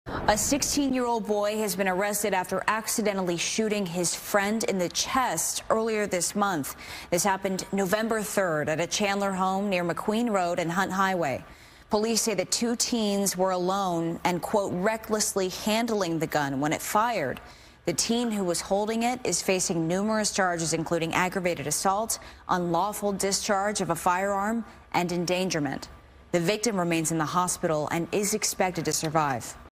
Chandler teen arrested for 'recklessly' shooting friend in the chest
A 16-year-old boy has been arrested after accidentally shooting his friend in the chest earlier this month.